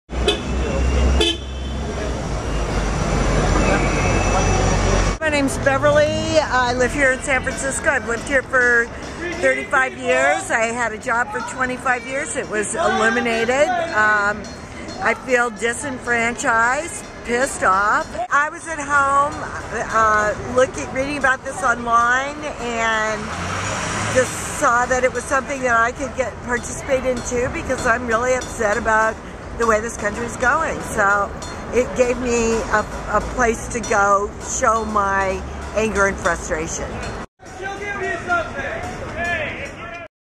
Beverly Best, 60, on Why She's Protesting With Occupy SF
Beverly Best outside the Federal Reserve Building in San Francisco in the early days of OccupySF

 By Queena Kim (Twttier: queenasookkim)